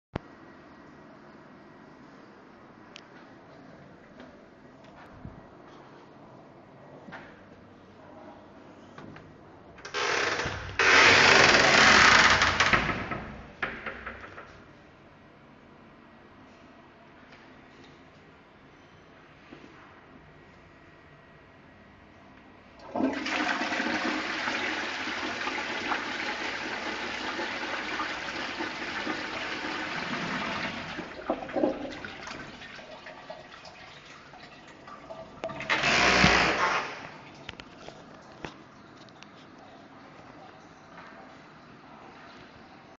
B+P College North Road Mechanics Building Toilets LL Shires
This is the college's mechanics building which was built in the 70s. There are 6 toilets all with low level Shires and 6 urinals and large wash basins. Only the cubicals have been replaced, everything else is original. I wish they had gone for high levels though as this setting would look briliant with high levels! The flush is standared for a low level.